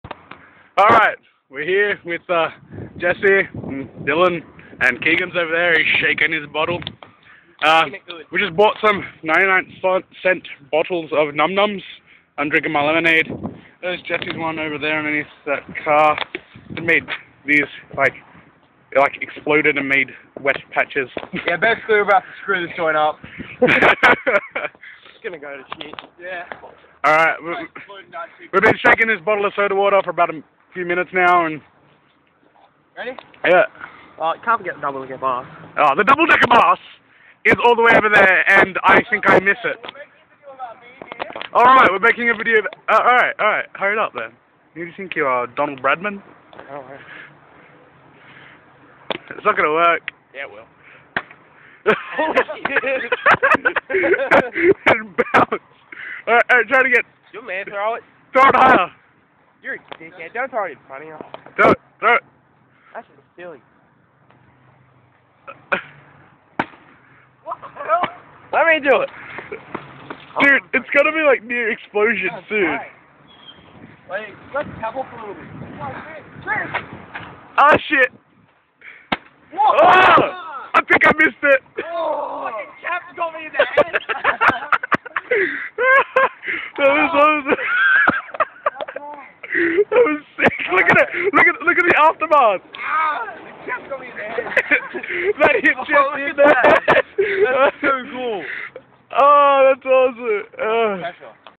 Soda bomb
Keegan tries but fails to set off a mini explosion so Jesse comes in and sets it off coz hes a champion.